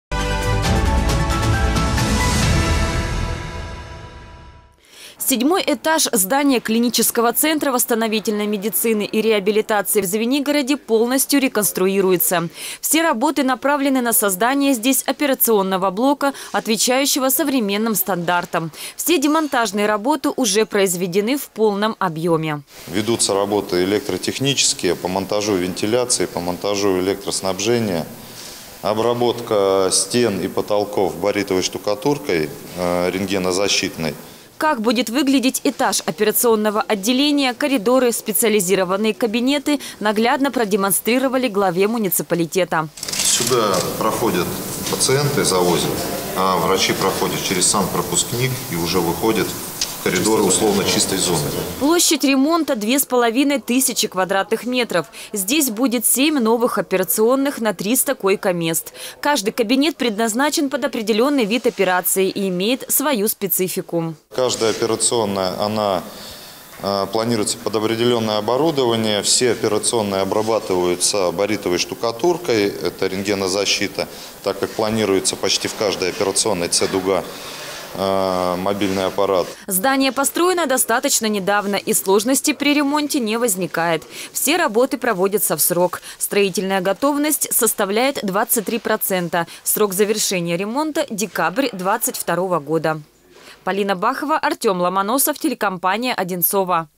Проверка ремонта горбольницы № 45
Глава округа Андрей Иванов вместе с главным врачом Одинцовской областной больницы Андреем Фадеевым проинспектировал ход ремонта операционных в Клиническом центре восстановительной медицины и реабилитации в Звенигороде. На каком этапе работы находятся сегодня?